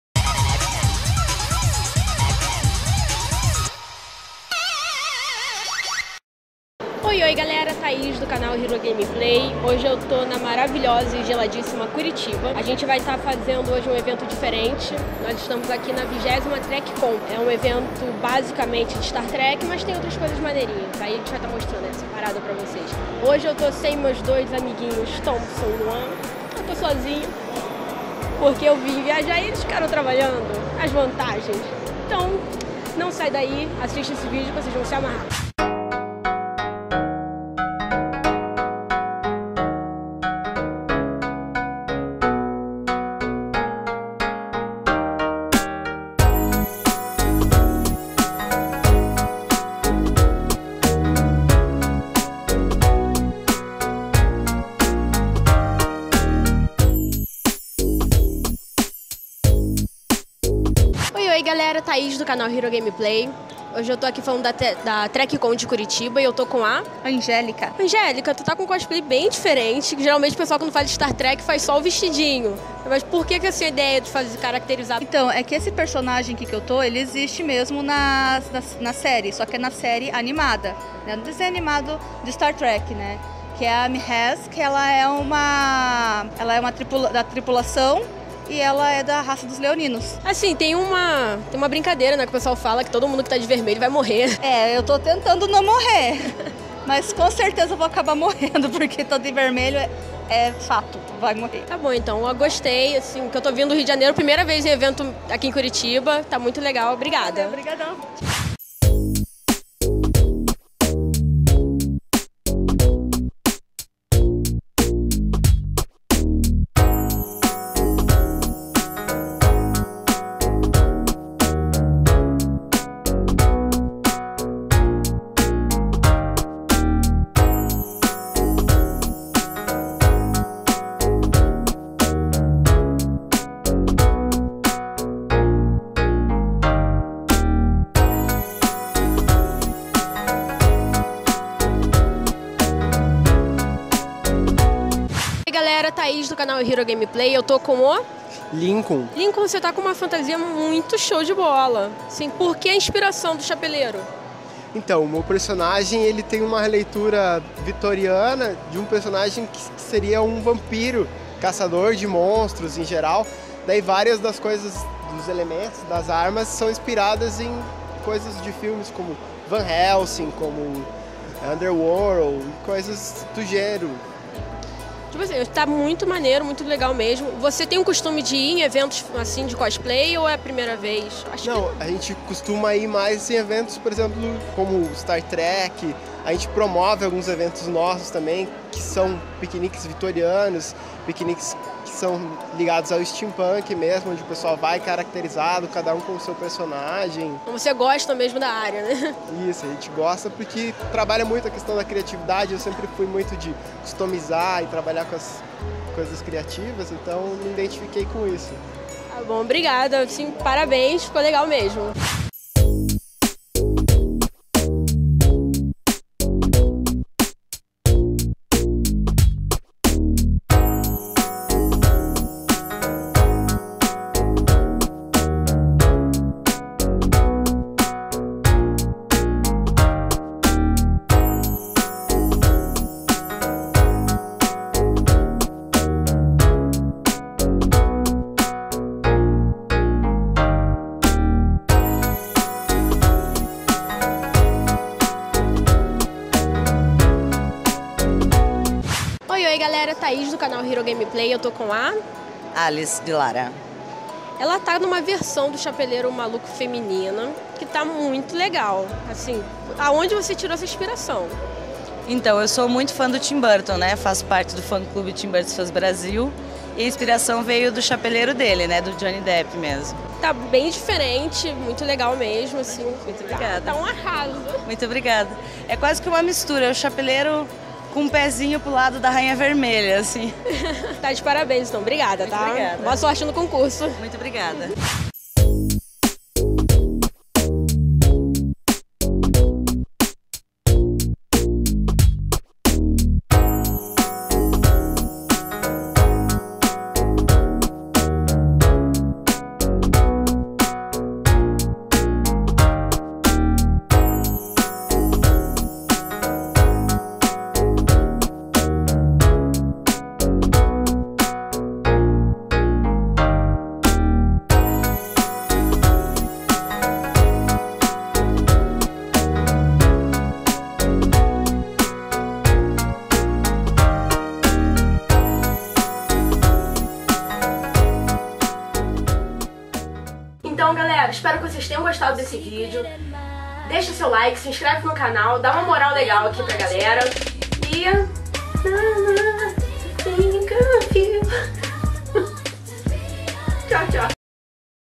STAR TREK - TrekCon Curitiba
20° TrekCon e Homeagem aos 50 anos de Star Trek.
Evento realizado no dia 30/06/2016 no Memorial de Curitiba, com a parceria do fã-clube Federação dos Planetas Unidos e a Fundação Cultural de Curitiba.

Contou com fãs e atividades para o púbico de StarTrek entre outras coisas muito legais!
CANAL HERO GAMEPLAY cobrindo o Evento no Paraná.